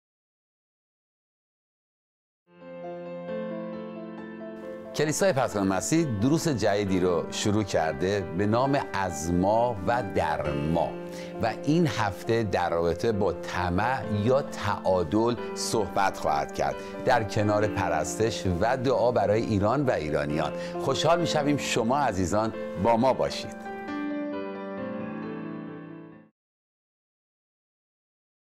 این یکشنبه با ما باشد در کلیسای پرستندگان مسیح
موضوع از ما و در ما